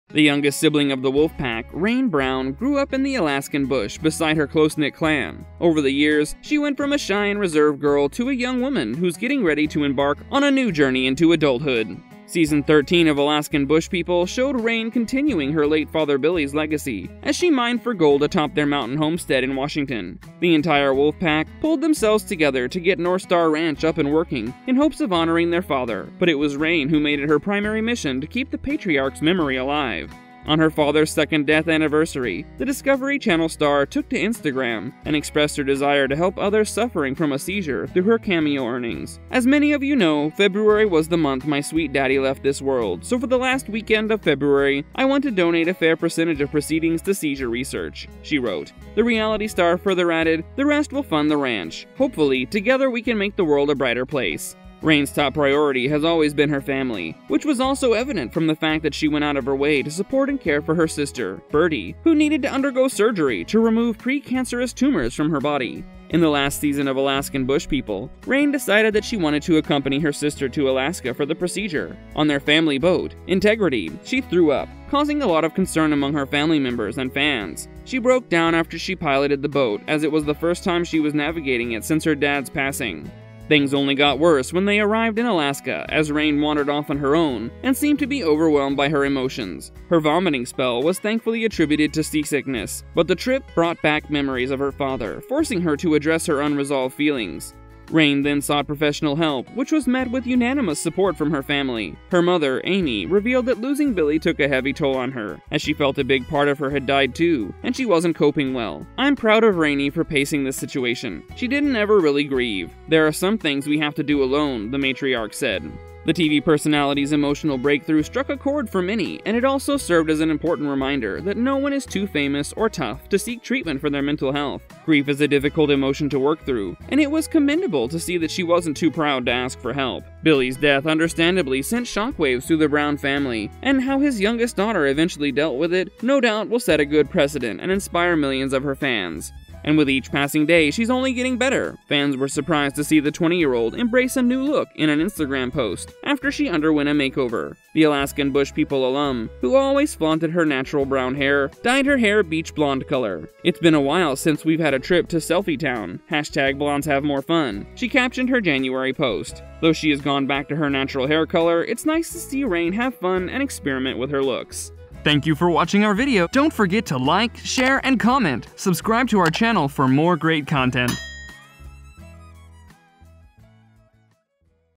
The Stunning Transformation of Rain Brown (Alaskan Bush People)
The youngest sibling of the ‘Wolf Pack’, Rain Brown grew up in the Alaskan Bush beside her close-knit clan. Over the years, she went from a shy and reserved girl to a young woman who is getting ready to embark on a new journey into adulthood.

Season 13 of Alaskan Bush People showed Rain continuing her late father - Billy’s legacy as she mined for gold atop their mountain homestead in Washington. The entire Wolf Pack pulled themselves together to get North Star Ranch up and working in hopes of honoring their father but it was Rain who made it her primary mission to keep the patriarch’s memory alive.